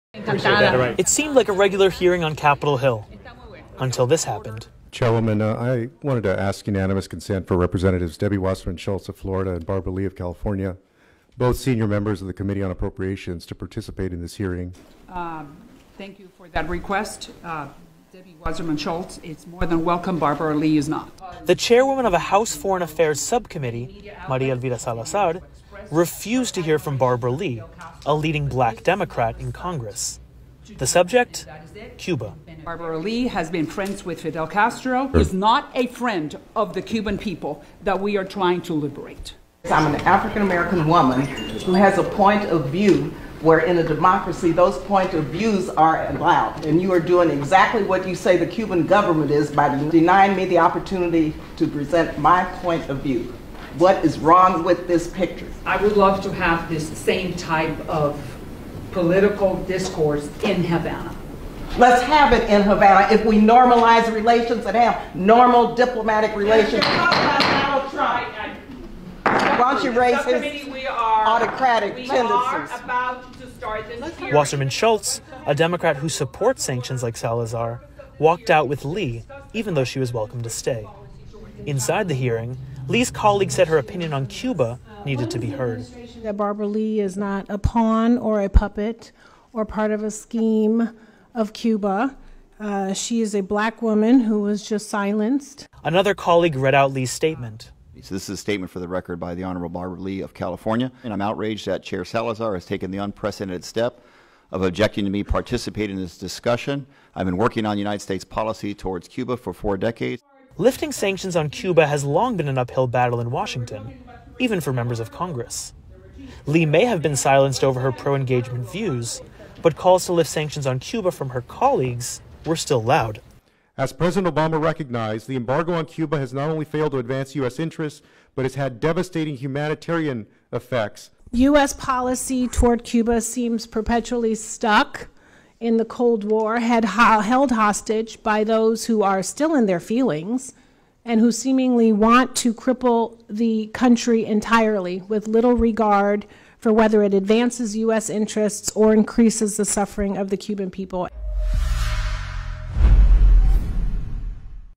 María Elvira Salazar enfrenta a congresista Barbara Lee por apoyar la lucha contra el bloqueo a Cuba
La senadora Maria Elvira Salazar cubanoamericana de línea dura le dijo a Congresswoman Barbara Lee que no podía unirse a una audiencia del Subcomité de Asuntos Exteriores de la Cámara de Representantes de USA sobre Cuba.

Salazar acusa al gobierno cubano de reprimir la libertad de expresión y hace lo mismo con Lee, una mujer negra que aboga por el levantamiento del embargo.